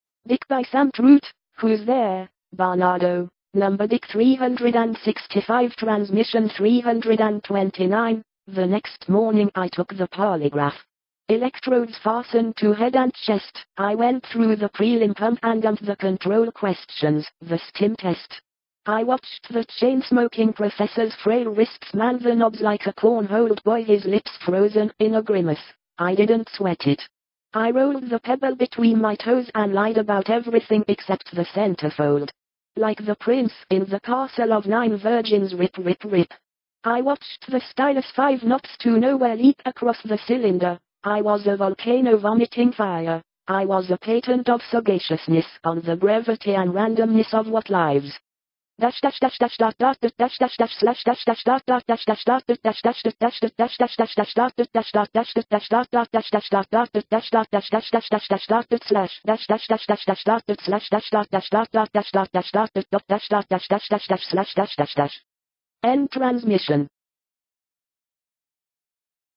DICK 329
This is one of 365 transmissions (22.Nov.12 - 22.Nov.13) of DICK, a book by Sam Truitt. For more see DICK: An Oblique Kennedy Conspiracy Countdown.